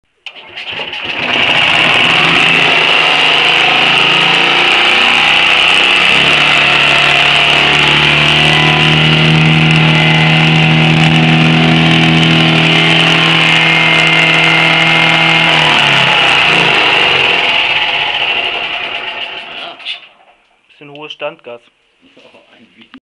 Heinkel Tourist 103 A1 - first run after 15 years
Kein Schlüssel, keine passende Batterie, aber Zündfunke und Sprit. Das reicht!